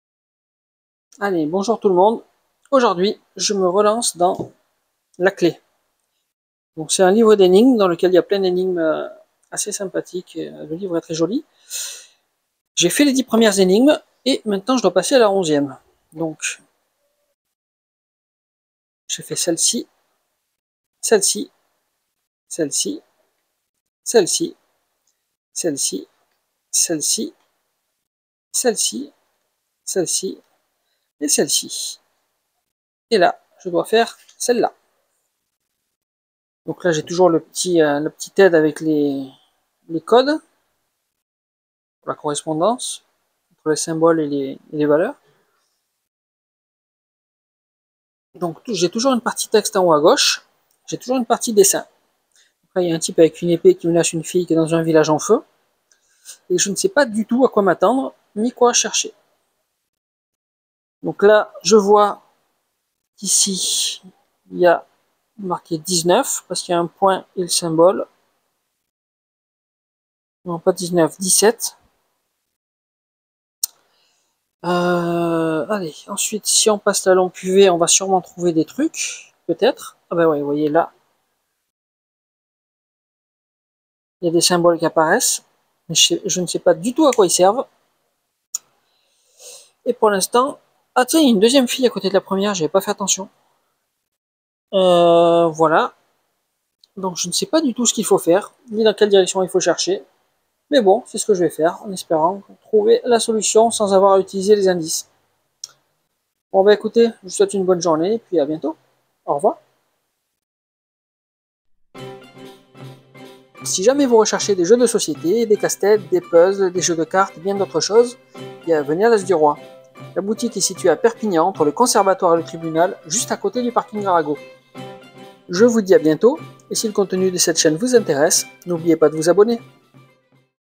Astolie - La clef - Énigme 10 - Livre d'énigmes
On jette un rapide coup d’œil sur l'énigme N°10 du jeu " La Clef ", un magnifique livre d'énigmes dans lequel on doit trouver et résoudre des énigmes toutes aussi originales qu'intéressantes et bien conçues.
On ne sait rien, on ne nous explique rien ... on doit totalement se débrouiller et chercher dans quelle direction aller.

Nota : Les règles du jeu expliquées dans cette vidéo sont les règles les plus importantes qui permettent de comprendre l'essence du jeu.
Pour avoir les règles exactes, merci de vous référer au livret des règles.

Type de jeu : Livre d'énigmes
Age : A partir 14 ans
Nombre de joueur : A partir de 1 joueur
Durée : 30 heures minimum

Boutique :
As du Roi - 49 Rue Maréchal Foch - 66000 Perpignan
Du Mardi au samedi : 10H00/12H30 - 14H00/19H00